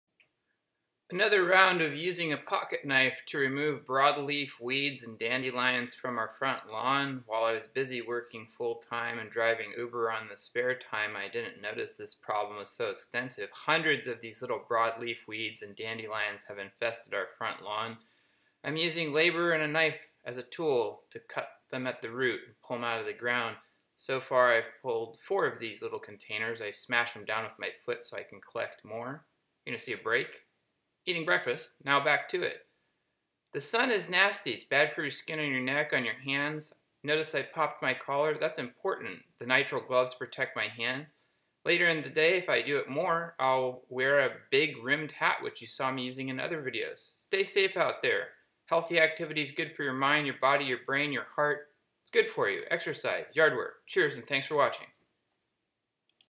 Speed Weeding round 4 with a pocket knife :) Any ideas for a petsafe herbicide to help me ?
Using a pocket knife I cut more weeds out of our front lawn. For a few months I was working full time & driving UBER on my days off & before & after work & failed to notice weeds infesting our front lawn. Multiple rounds of weeding, captured with timelapse video are documenting & creating evidence of the effort I put into dealing with the problem.

Unable to dedicate myself to this, I am researching herbicides & looking for a pet safe biodegradable eco-safe herbicide for lawn that works against broadleaf weeds & dandelions. If you have any suggestions about a safer herbicide to use, for my neighbors cats & dog's safety, they walk around in my yard all the time / I hope to avoid applying something that could get on their paws & indirectly poison or harm them. Any useful suggestion about a pet safe herbicide for lawns would be greatly appreciated.